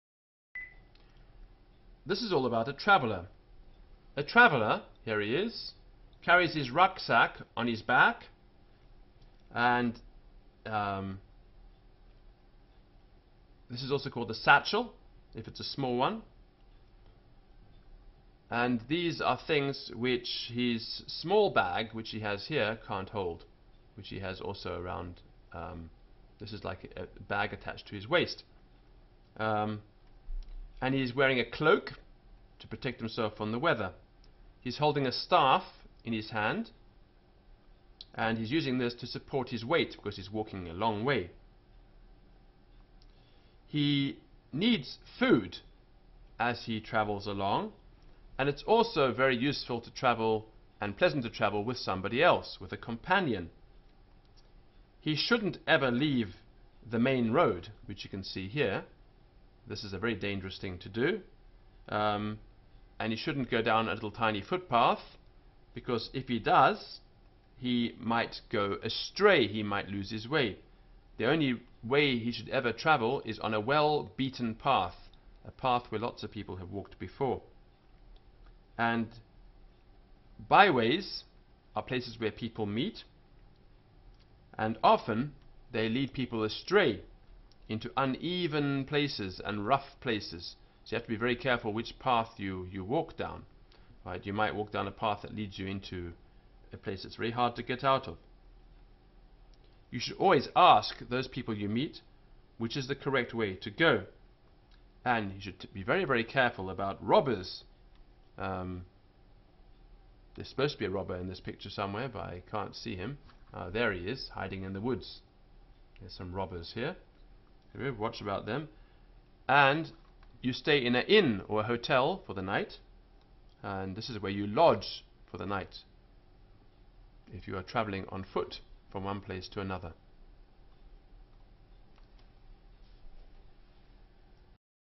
084 The World in Pictures The Traveller
Comenius recommended that a student should be familiar with the vernacular text of his Vestibulum, Orbis and Janua. The following readings go through the chapters in English.